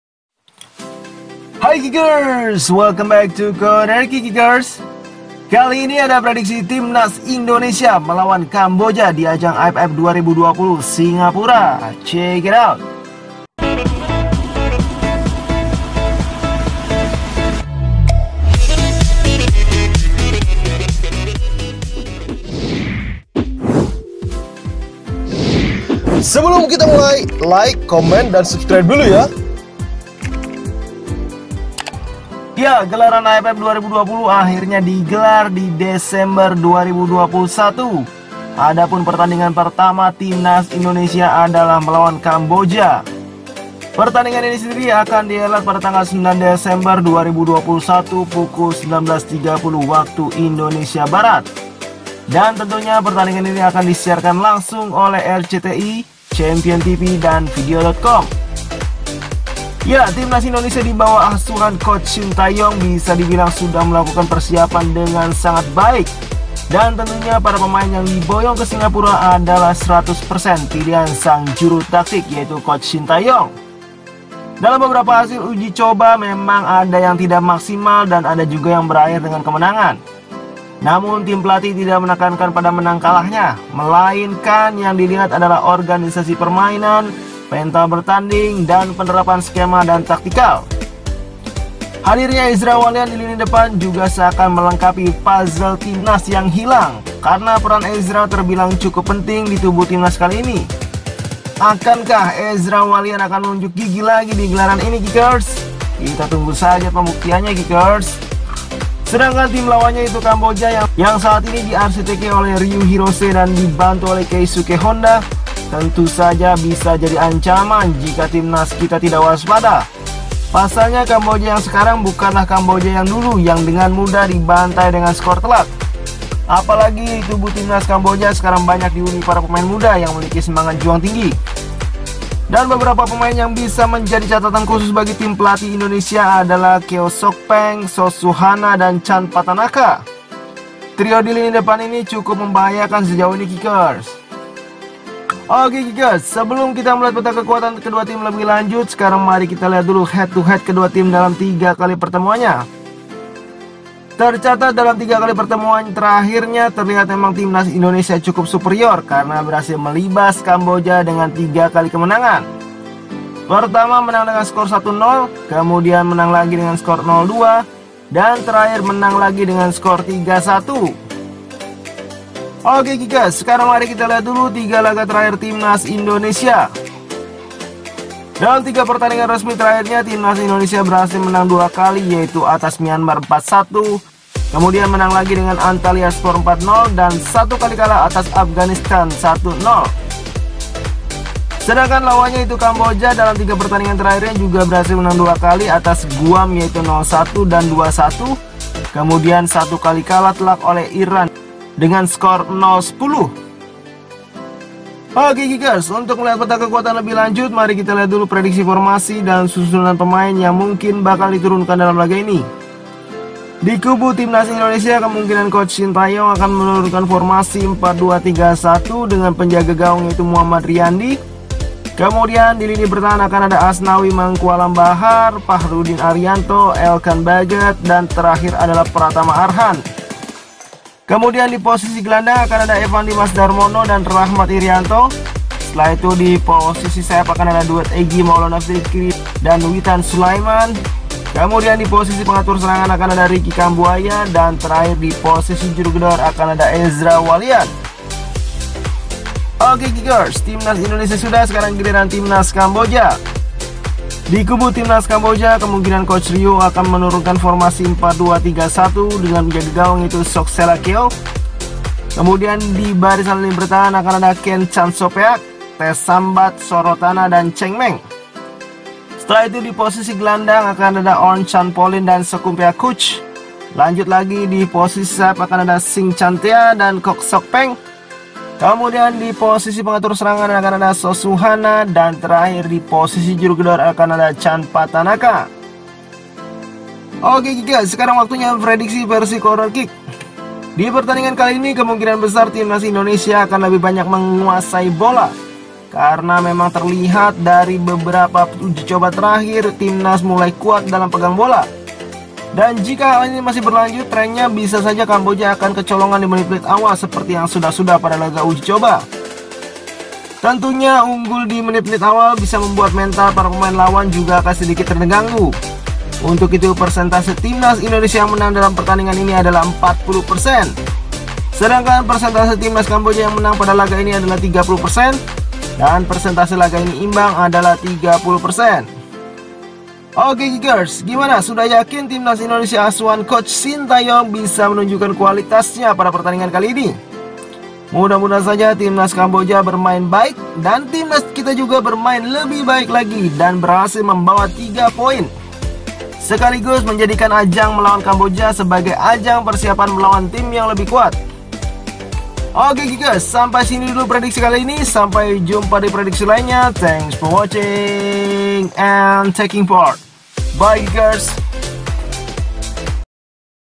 LIVE RCTI - Prediksi Indonesia VS Kamboja, AFF Cup 2020, 9 Desember 2021
Ya gelaran aff 2020 akhirnya di gelar di desember 2021. Adapun pertandingan pertama timas indonesia adalah melawan kamboja Adapun pertandingan ini akan di helat pada tgl 9 desember 2021 pukul 19.30 wib. Dan tentunya pertandingan ini akan disiarkan langsung oleh rcti, champion tv dan vidio.com

Ya timnas indonesia dibawah asuhan coach sty, bisa dibilang sudah melakukan persiapan dengan sangat baik dan tentunya para pemain yg diboyong ke singapura adalah 100 persen pilihan sang juru taktik yaitu sty, dalam beberapa hasil ujicoba memang ada yg tidak maksimal dan ada juga yg berakhir dengan kemennangan, namun tim pelatih tidak menekankan pada menang kalahnya, melainkan yg dilihat adalah organisasi permainan, mental bertanding, dan penerapan skema dan taktikal. Hadirnya ezra wallian di lini depan juga seakan melengkapi puzzle timnas yg hilang karna peran ezra terbilang cukup penting di tubuh timnas kali ini, akankan ezra akan unjuk gigi lagi pada gelaran ini?

Sedangkan tim lawannya yaitu kamboja, yg saat ini di arsiteki oleh ryu hirose dan dibantu oleh Keisuke honda, tentu saja bisa jadi ancaman jika timnas kita tidak waspada, pasalnya kamboja yg skr bukan lah kamboja yg dulu yg dengan mudah dibantai dengan skor telak, apalagi di tubuh timnas kamboja skr banyak dihuni para pemain muda yg memiliki semangat juang tinggi, dan beberapa pemain yg bisa menjadi catatan khusus bagi tim pelatih adalah keo shokpeng, sos suhana, dan chan vatanaka. Trio di lini depan ini cukup membahayakan sejauh ini kikers.

Corner Kick berkomitmen untuk senantiasa memberikan informasi bursa transfer, jadwal, prediksi, hasil pertandingan dan klasemen sepak bola nasional